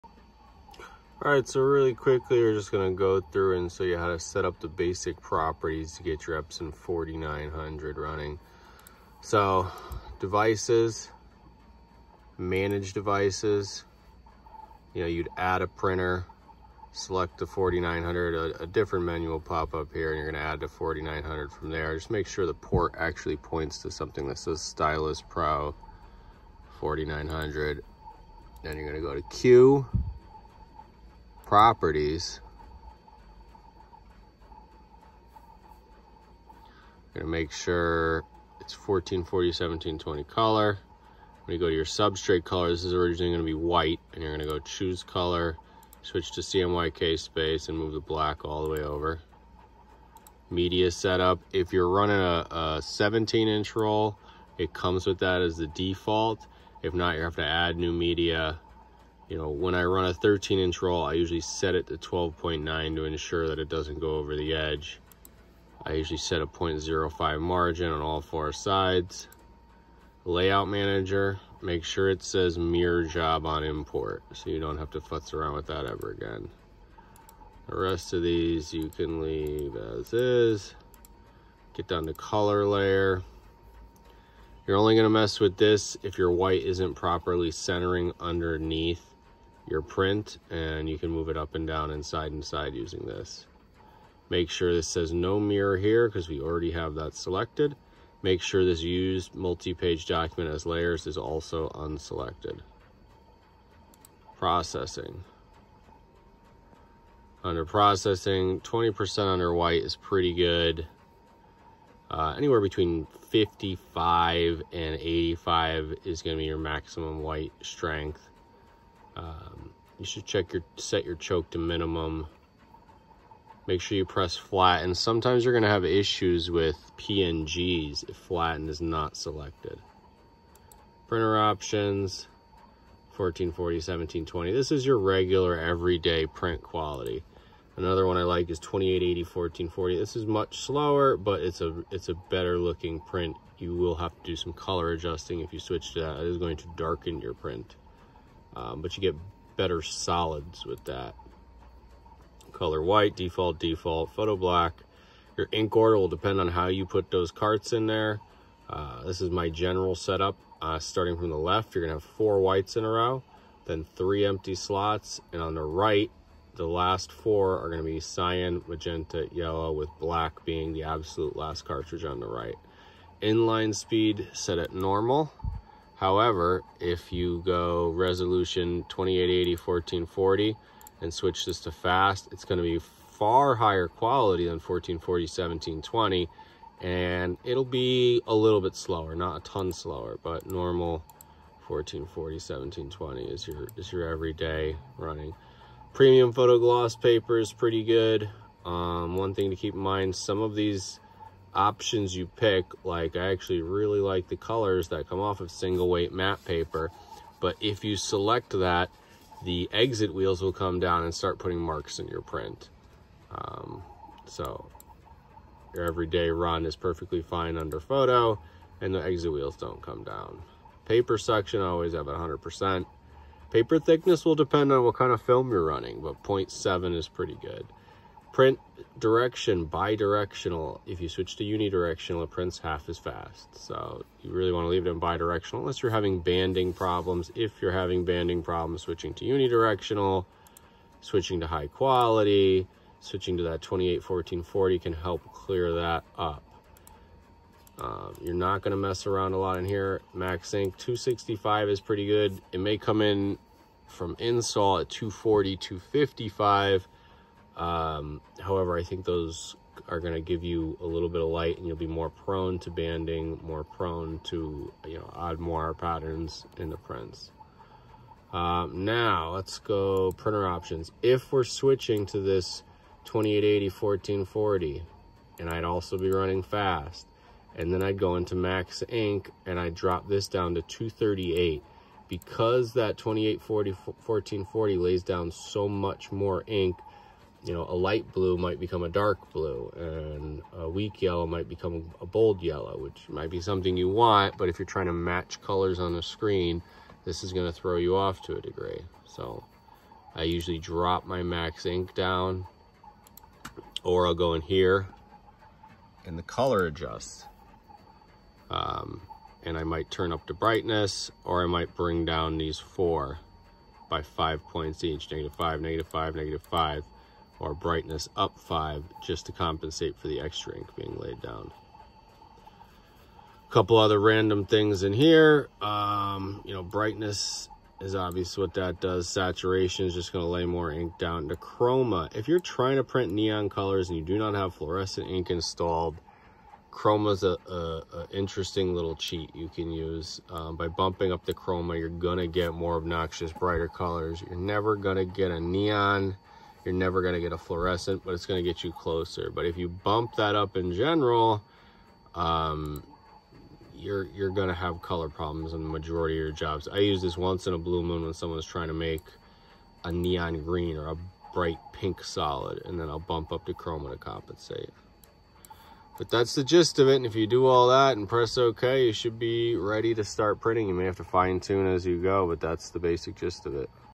Cadlink Digital Factory Basic Setup Epson 4900
Follow the on screen instructions to get your Epson 4900 started today